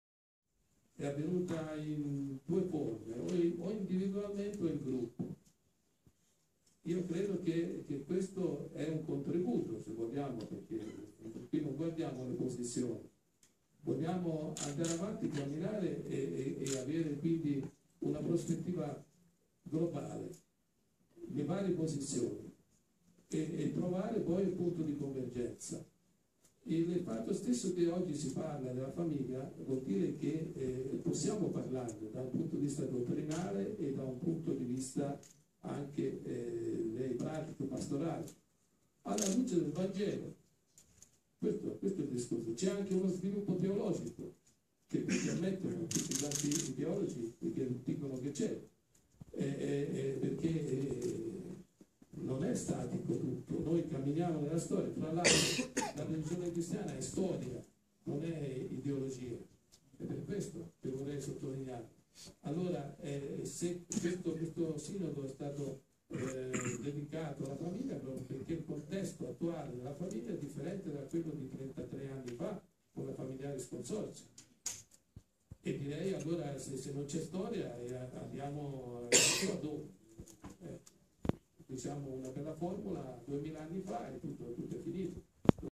Sinodo sulla famiglia. Card. Baldisseri: vogliamo trovare un punto di convergenza
Non guardiamo le posizioni, guardiamo ad andare avanti, a camminare e avere una prospettiva globale. Tra le varie posizioni trovare un punto di convergenza. Il fatto stesso che oggi si parla della famiglia vuol dire che possiamo parlarne da un punto di vista dottrinale e da un punto di vista pratico-pastorale, alla luce del Vangelo. C’è anche uno sviluppo teologico. Questo Sinodo è stato dedicato alla famiglia proprio perché il contesto attuale della famiglia è diverso da quello di 33 anni fa, epoca della “Familiaris Consortio”.